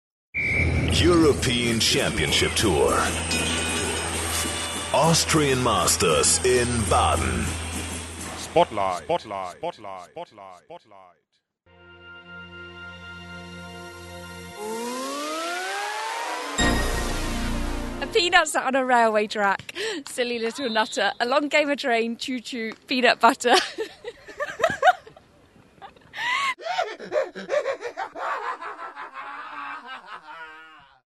The Austrian Masters 2009_joke of the day Friday
One clip of the austrian masters of the eurobeachtour 2009. This time: Joke of the day - friday